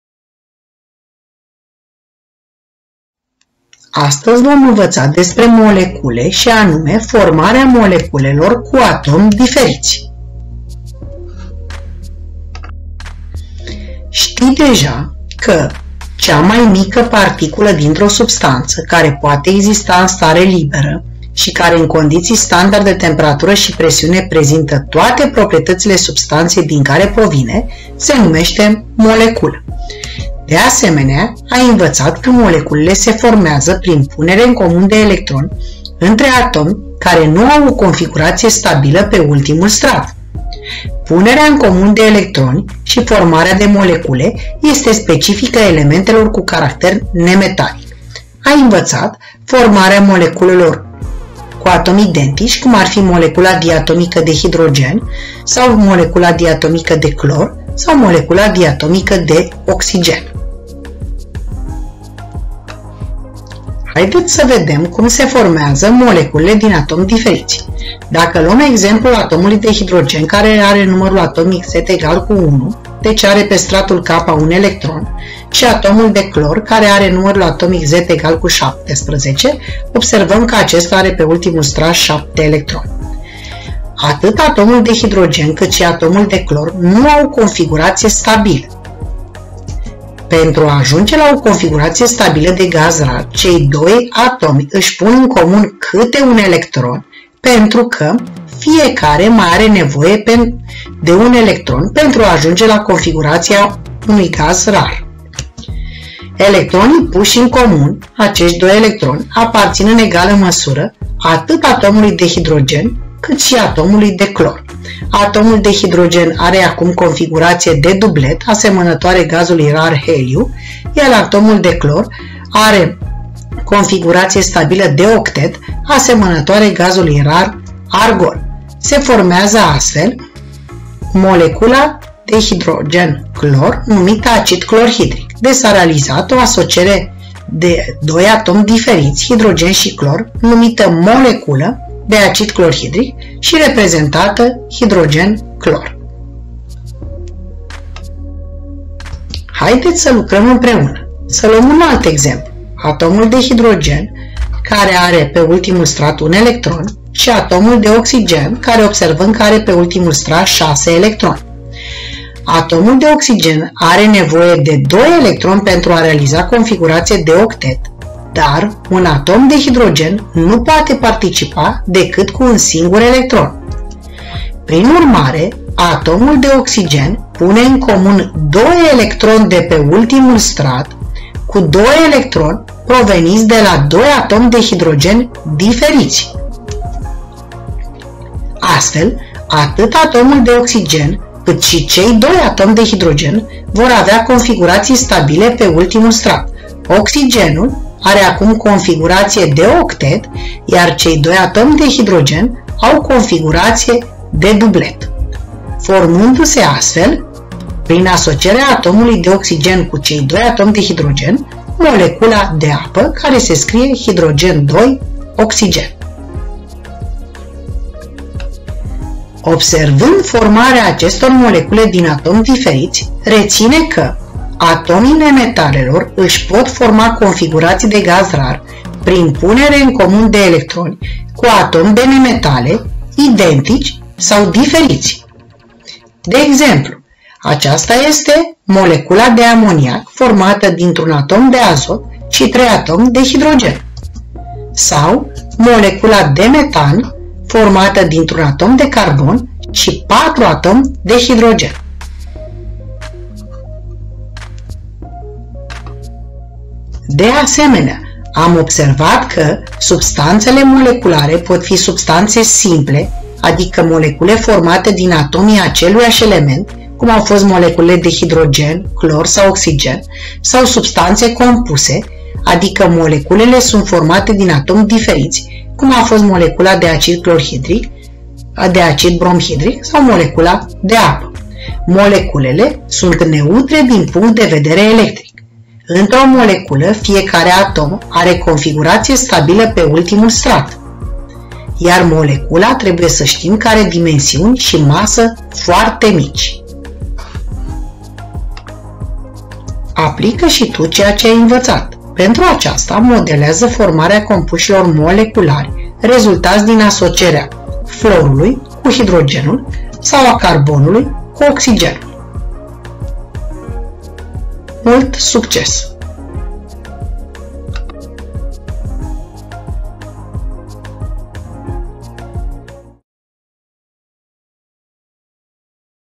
Molecule cu atomi diferiți
Resursa de tip video propusă conține informații referitoare la formarea unor molecule, și anume molecule cu atomi diferiți. Este prezentată prezentată formarea moleculelor de acid clorhidric (HCl) și apă (H 2 O).
Această resursa poate fi utilizată la clasă 8 – 25 minute.

Autor: Madalina Angelusiu